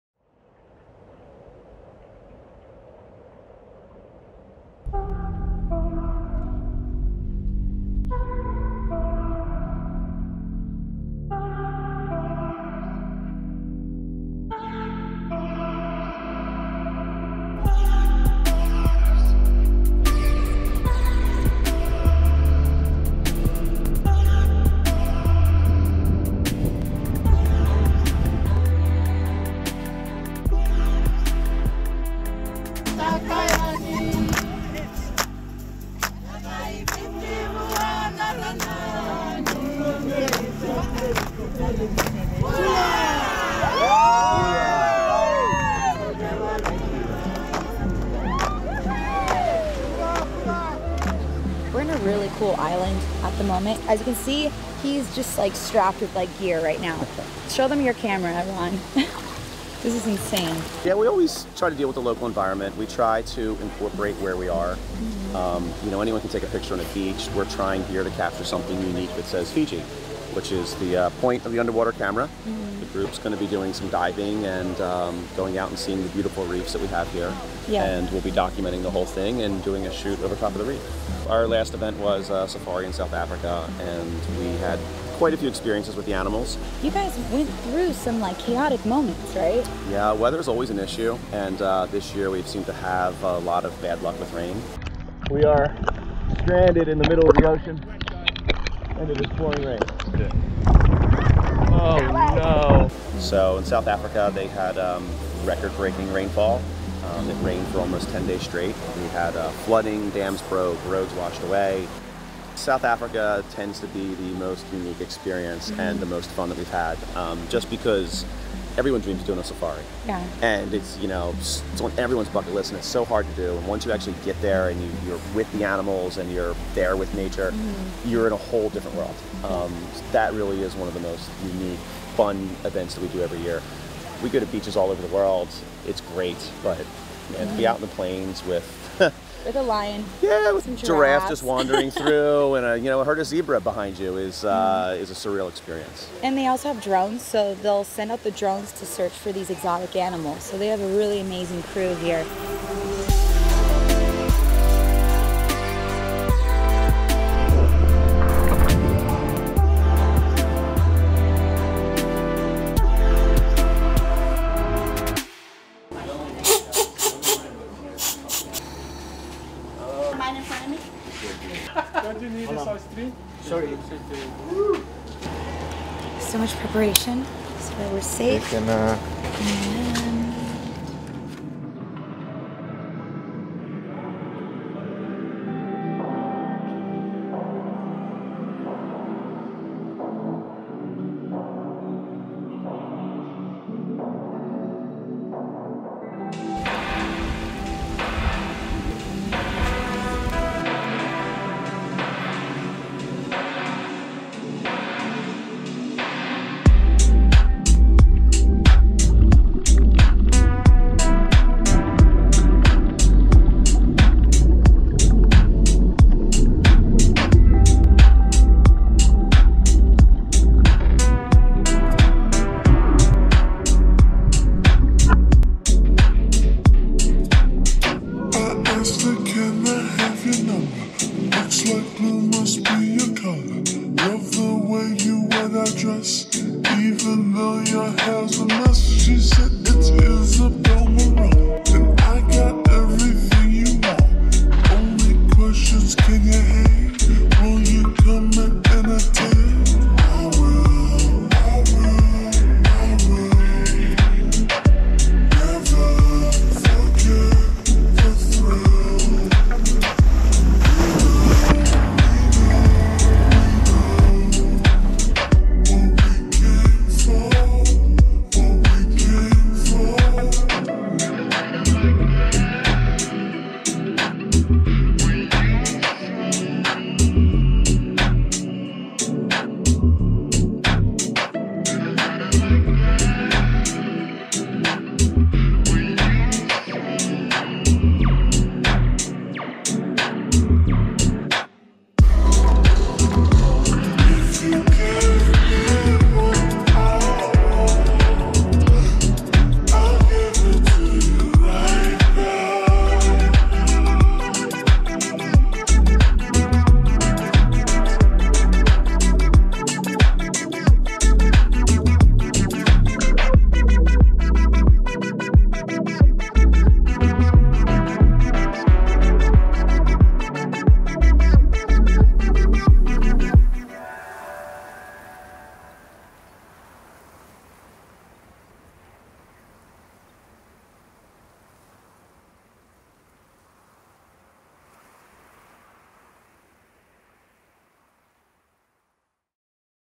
A.P.E. Fiji SCUBA
Association of Photo Events attendees take some time off from photoshoots to get PADI SCUBA certified and enjoy the reefs of Fiji's Beqa Lagoon area.